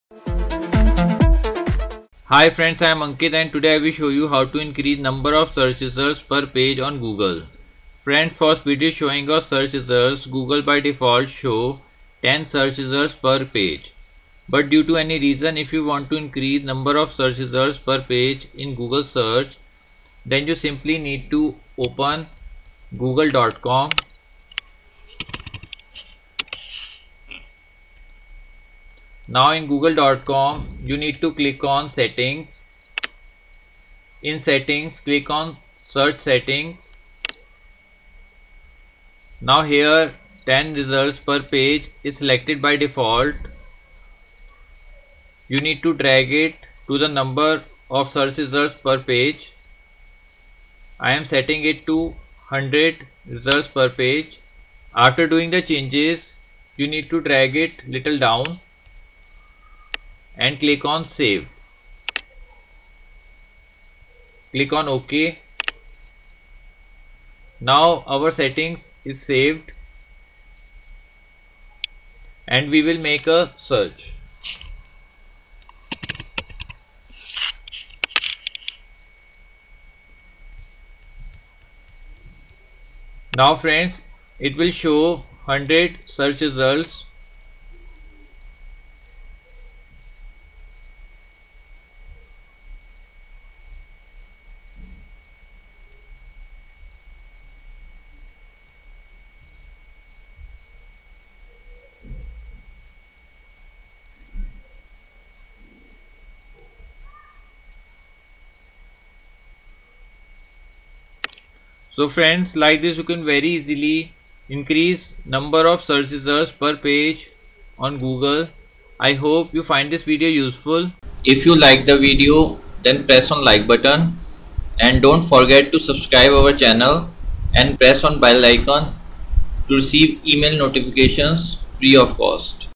How to Increase Number of Search Results Per Page on Google
In this video we have shown How to Increase Number of Search Results Per Page on Google Step by Step